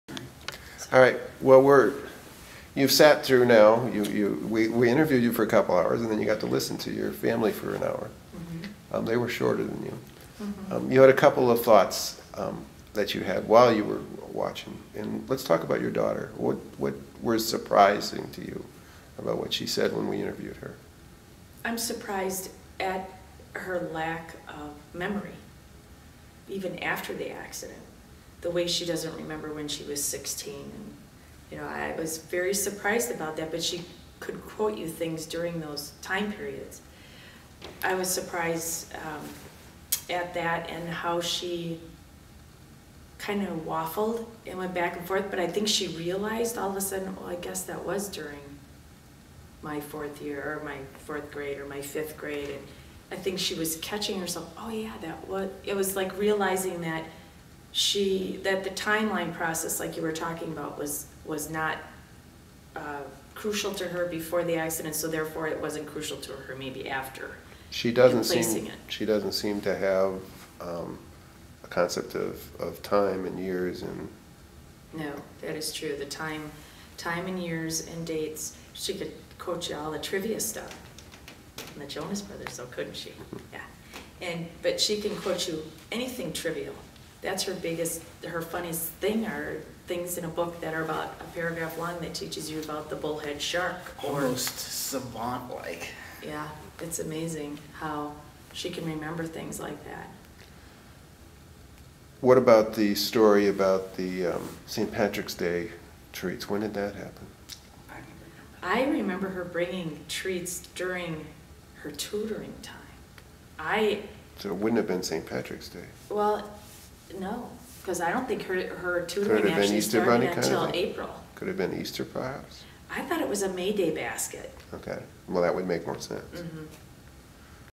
Mom On Nancys Time Memory After Brain Injury
This video was made for our TBI Voices project.  We interviewed thirty real life survivors of brain injury.  In these interviews we talked to the survivors about how they became brain injured and who they were before their brain injury.  We also talk about their recovery and what deficits they have encountered .
TBI Attorney Gordon Johnson conducts interview of brain injury survivor.
I am surprised at her lack of memory...even after the accident.  I was surprised how she went back and forth and tried to remember.  The timeline was not crucial to her.  She doesn't seem to have a concept of time and years and dates.  She could quote you trivia.  It is amazing how she can remember trivia.